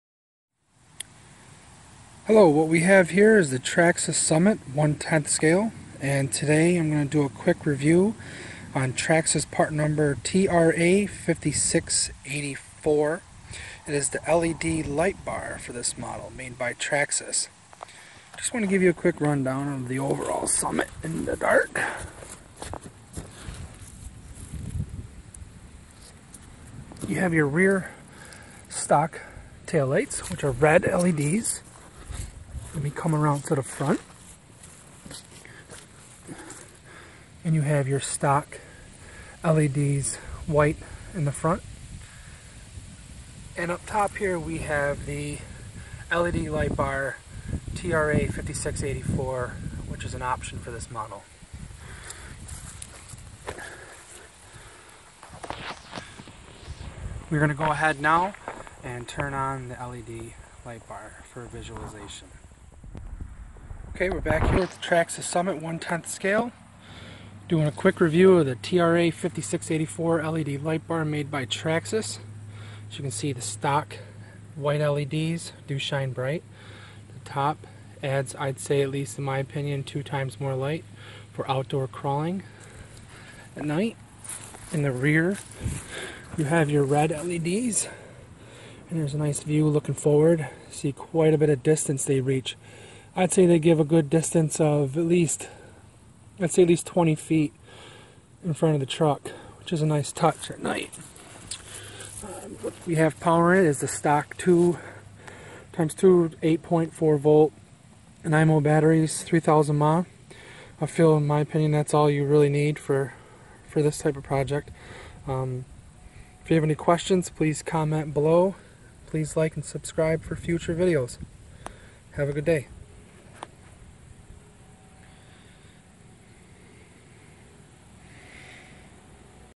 Traxxas Summit 1/10 scale Tra5684 Lightbar Review
Just a quick video to showcase the Traxxas Lightbar tra5684 mounted on a Summit 1/10 scale. If you have any questions or comments please say so below. BashOn!